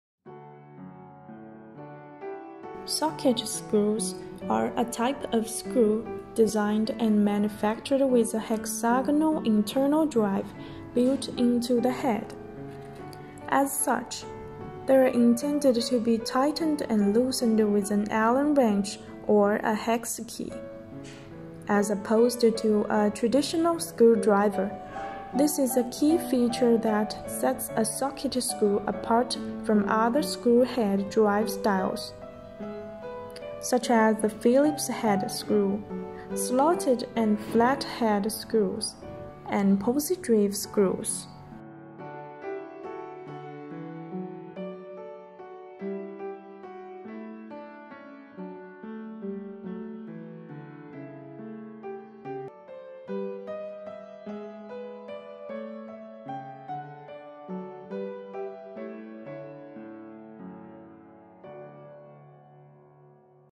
(HD) Stainless Steel Hex Socket Screws Full Details
Socket screws are a type of screw designed and manufactured with a hexagonal internal drive built into the head. As such, they are intended to be tightened and loosened with an allen wrench or hex key, as opposed to a traditional screwdriver. This is the key feature that sets a socket screw apart from other screw head drive styles such as the Phillips head screw, slotted and flat head screws, and pozidriv screws.

Hex socket screws are widely used in different fields: door&window system, building construction, gas and oil industry, high railway and electronic communication, new energy, automotive industry and so on.

We are capable to provide you OEM&ODM service.